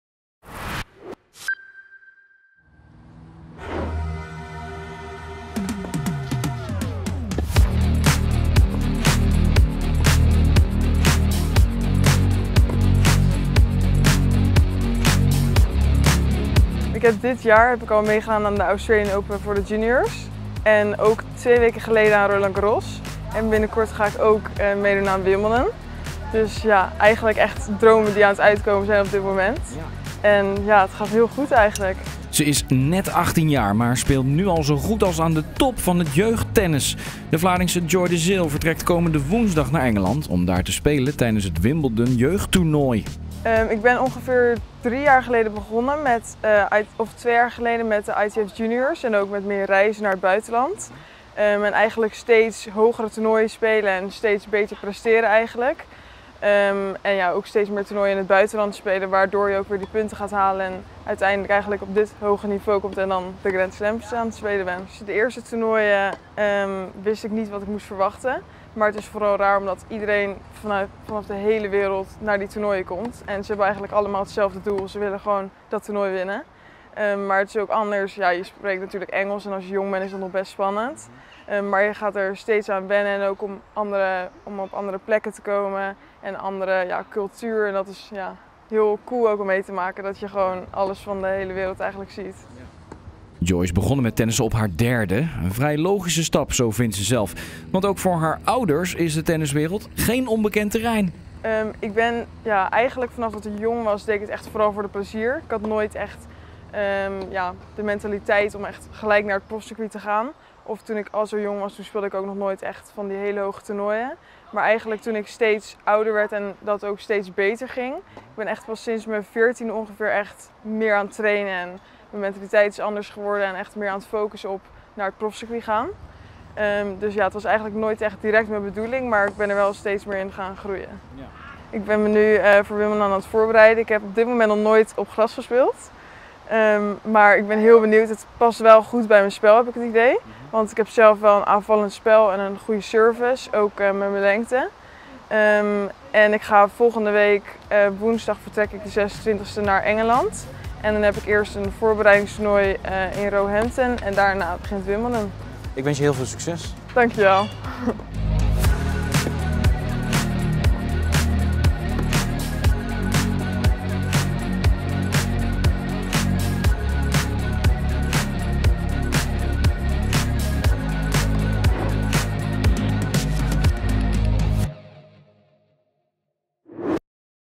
Vlaardings tennistalent Joy de Zeeuw (18) naar Wimbledon: ‘past goed bij mijn spel’
De Vlaardingse Joy de Zeeuw (18) is hard op weg om een internationale tennistopper te worden. De Zeeuw speelde dit seizoen al op de jeugdtoernooien van de Australian Open en Roland Garros. In juli doet ze mee aan de juniorversie van het befaamde Wimbledon in Engeland.